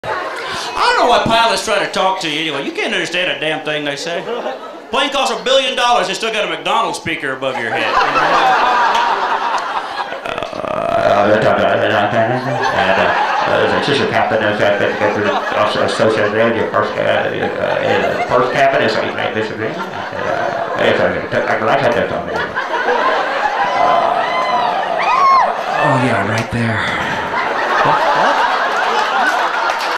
Steve McGrew: Pilots on Planes
Steve McGrew is a local Denver comic and also a radio personality. To learn more and purchase tickets for Steve go